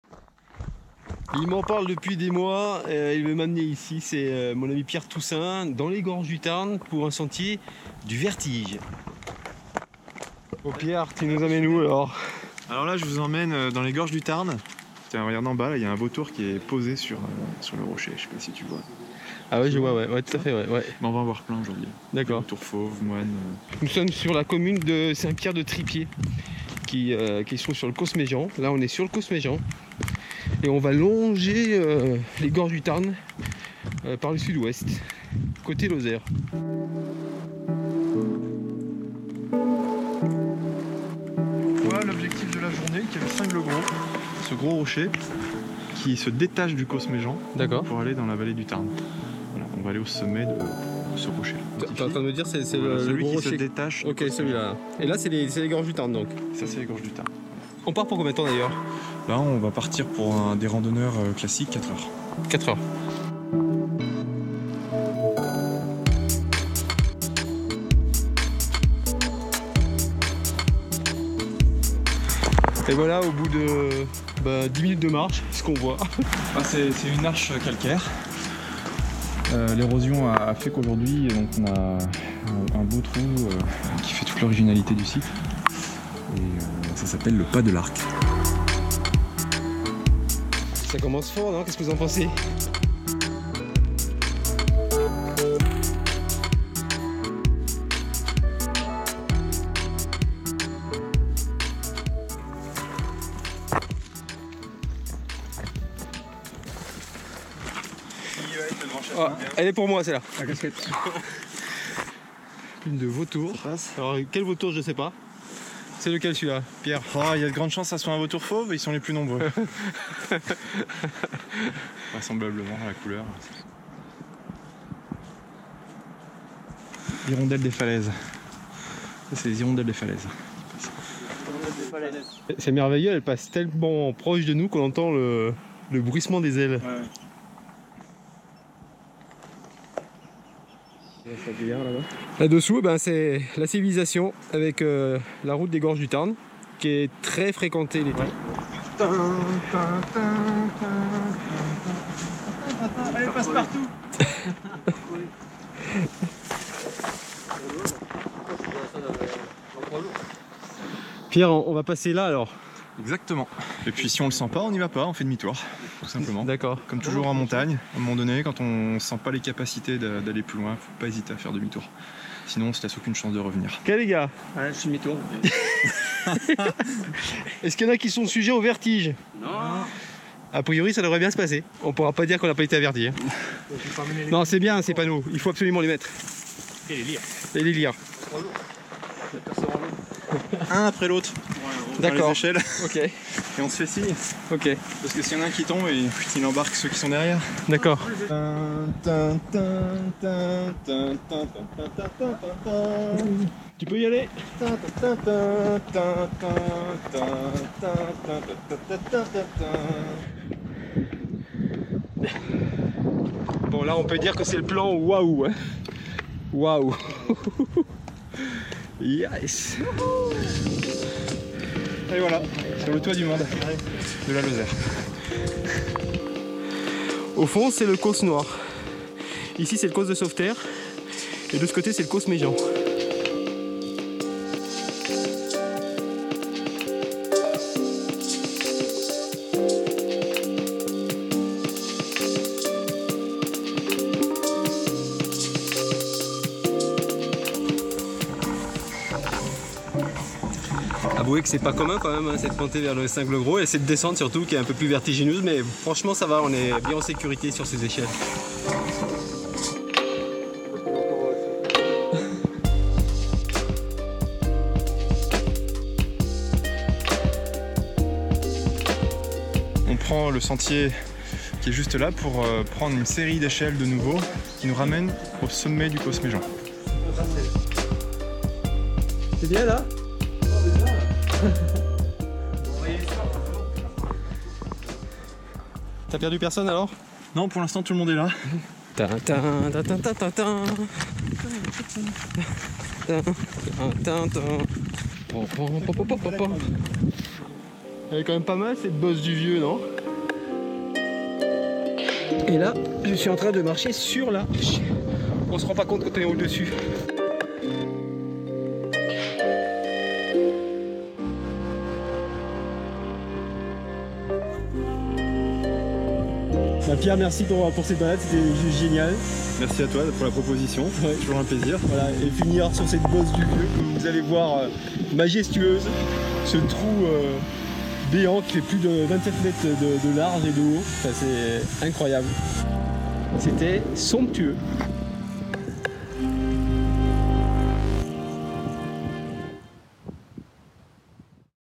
Rando bien zinzin au dessus des Gorges du Tarn : le rocher de Cinglegros
C'est la rando la plus vertigineuse de la région : l'escalade du Rocher de Cinglegros par les echelles, avec vue imprenable sur les Gorges du Tarn, 400 m au dessous. Ce belvédère, survolé par les vautours, n'est pas la seule curiosité de cet itinéraire engagé. Vous traverserez aussi la falaise par le Pas de l'Arc puis vous trônerez au dessus d'un trou géant, la Baus Del Biel, la bosse du vieux. Un itinéraire à retrouver sur mon Strava Zinzin Reporter au 24 septembre 2018. Cette balade de 10 km pour 700 D, au départ de la Bourgarie sur le Causse Méjean,  est très technique. Elle est à reservée aux trailers qui n'ont pas le vertige ! J'ai été guidé sur ces chemins par mon pote ultra trailer, Pierre Toussaint, également organisateur de trail.